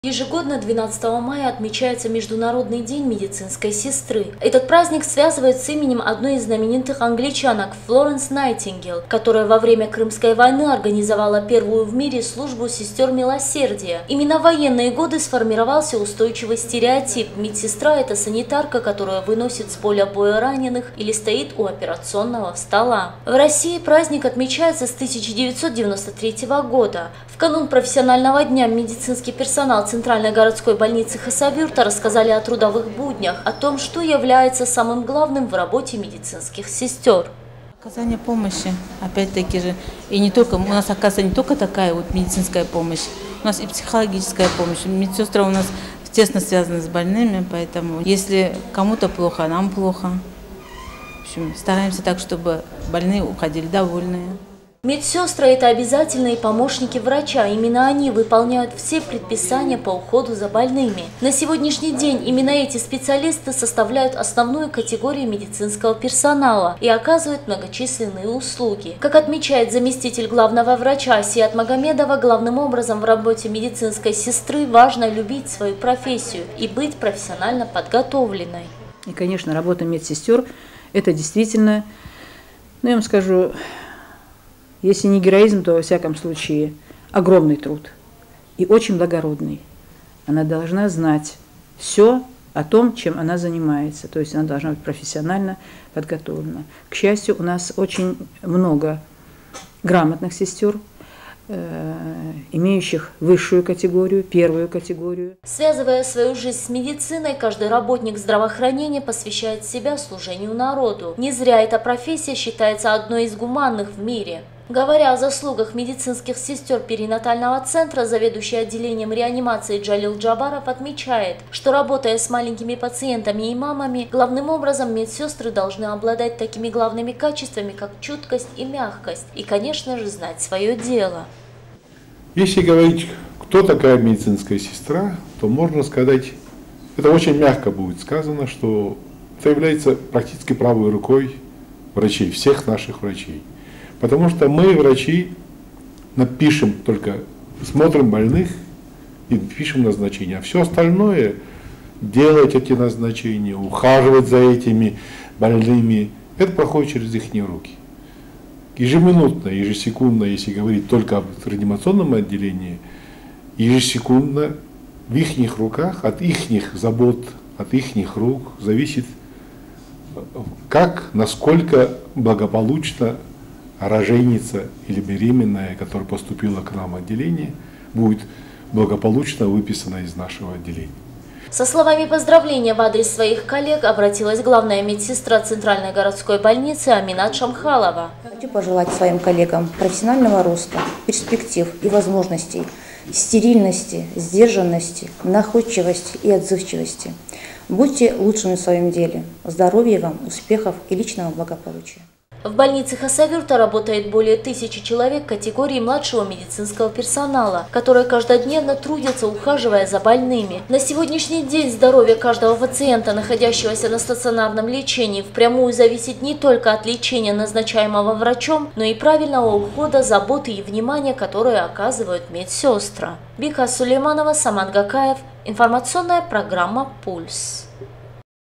Медсестры больницы Хасавюрта отмечают профессиональный праздник
Ежегодно, 12 мая отмечается Международный   день медицинской  сестры. В России  праздник отмечается с 1993 года. Медсестра – это обязательные помощники врача, именно они выполняют все предписания по уходу за больными.  В канун профессионального дня мед персонал ЦГБ Хасавюрта  рассказал о трудовых буднях, о том что является самым главным в работе медицинских сестер.